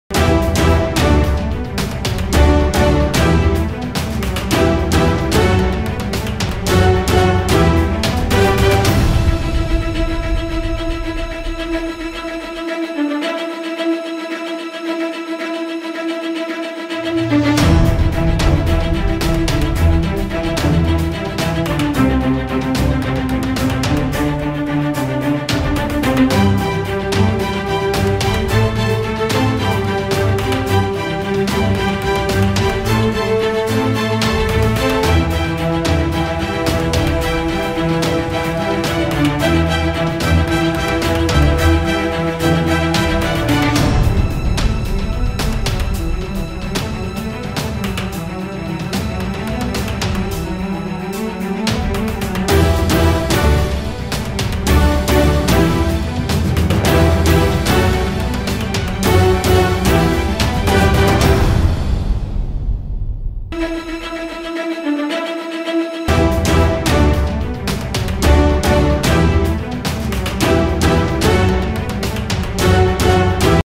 Mondiali 2014 Giocatori Brasile - Video Statistiche
Video con Statistiche e i giocatori del Brasile convocati per i Mondiali 2014.